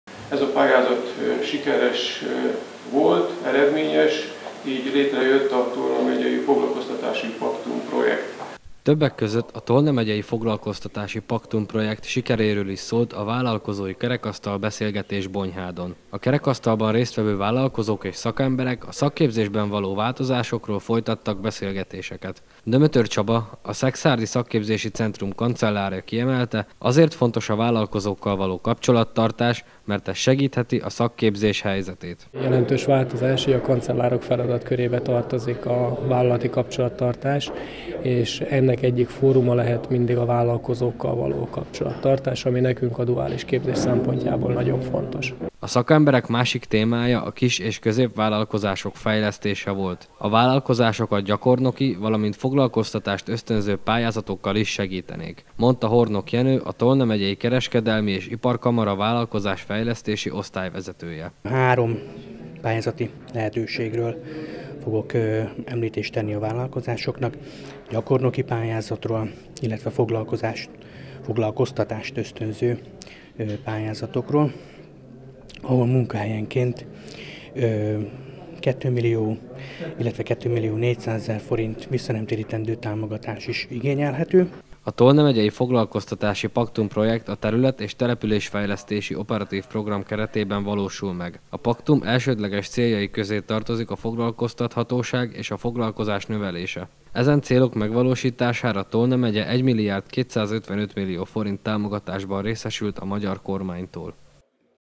Gyakornoki programmal segítenék a vállalkozóka 20190509
Gyakornoki programmal segítenék a vállalkozókat - hangzott el azon a kerekasztal beszélgetésen, amit Bonyhádon tartottak. A Tolna Megyei Foglalkoztatási Paktum Projekt keretében a munkahelyek, és a vállalkozások fejlesztéséről beszélgettek a résztvevő szakemberek és vállalkozók.